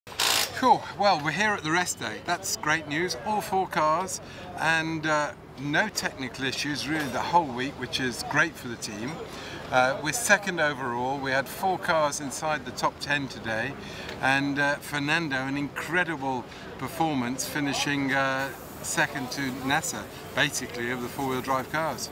Glyn Hall
Lack of dunes and fast terrain was against the team today.